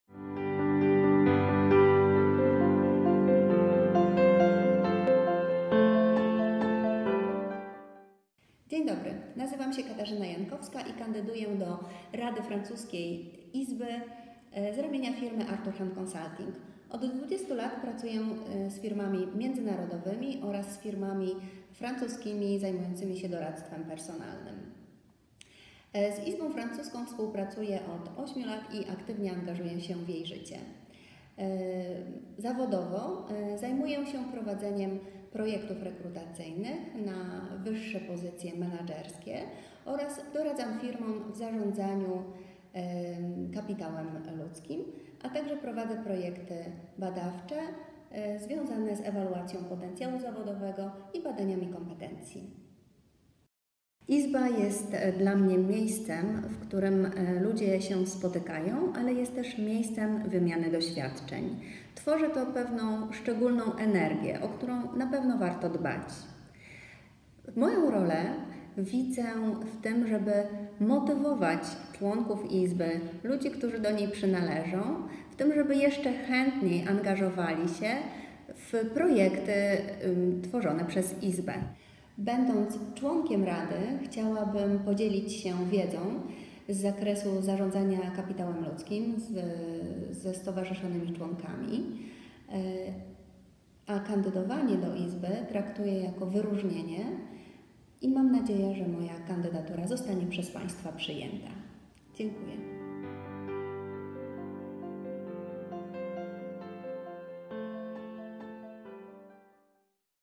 Katarzyna Jankowska - Kandydatura do Rady CCIFP / La candidature au Conseil de la CCIFP
Walne Zgromadzenie CCIFP / l'Assemblée Générale de la CCIFP - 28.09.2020
Wybory do Rady CCIFP 2020 / Les élections du conseil de la CCIFP 2020